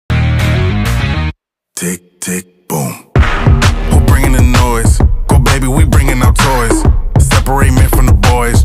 Hand Painted Squig Shirt Tie Dye Warhammer WAAAGH Orks
I'm often paintingwarhammer on shirts with beeswax & procion dyes @ DyeingToBatik ⁣⁣⁣⁣⁣⁣⁣⁣⁣⁣⁣⁣⁣⁣⁣⁣⁣⁣⁣on Instagram